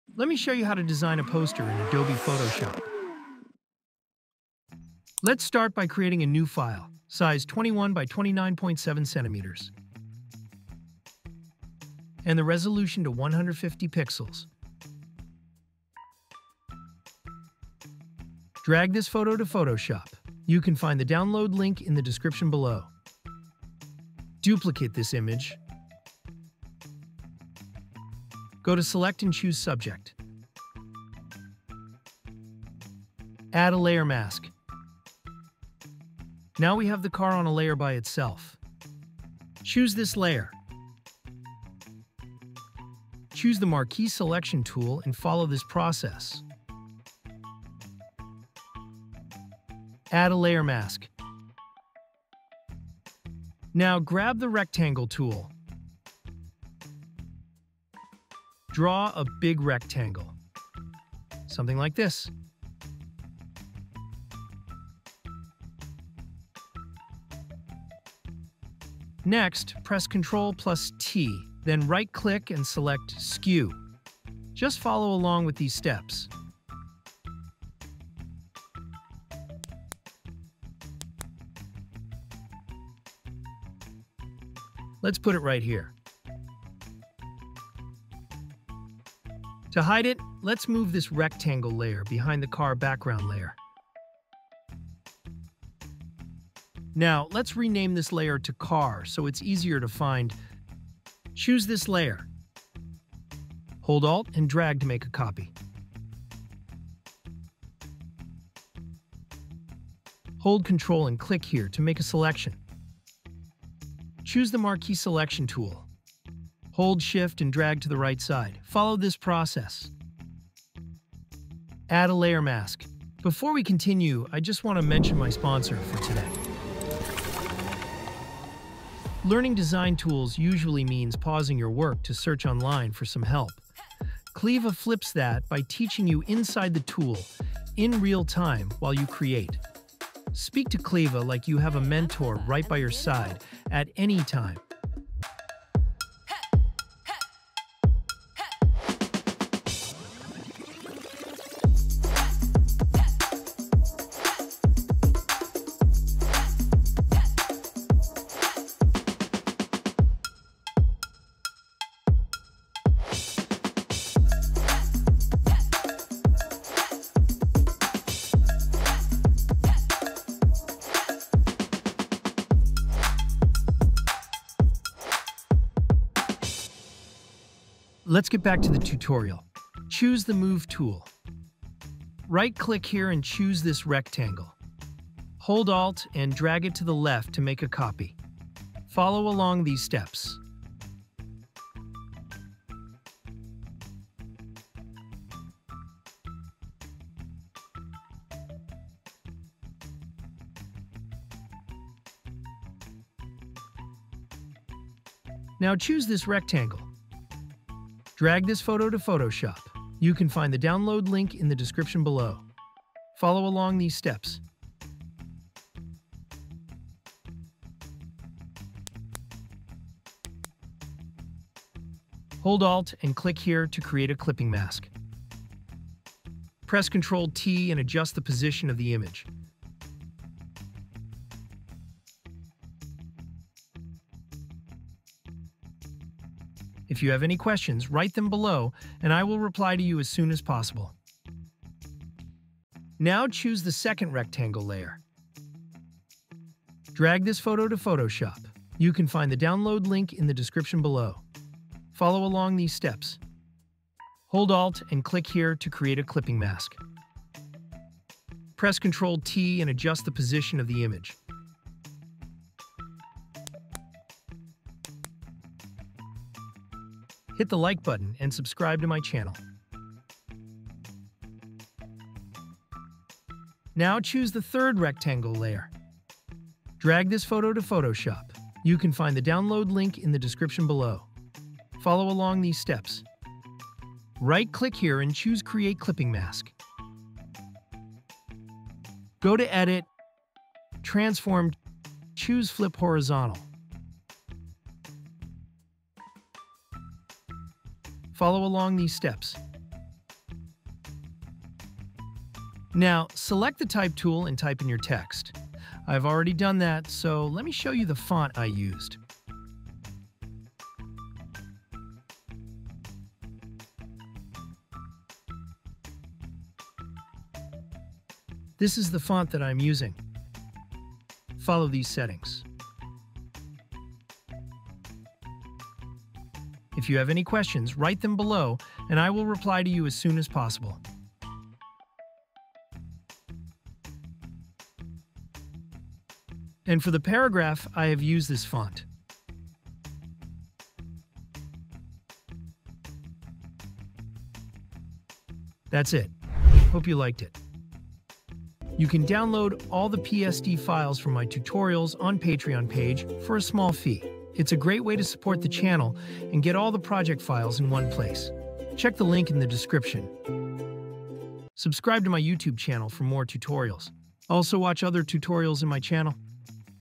Poster Design in Photoshop for Beginners!!!
Cleva helps creators learn complex design tools while they’re actually creating. Join the waitlist now:

In this Video, I’ll show you how I designed this poster in Adobe Photoshop.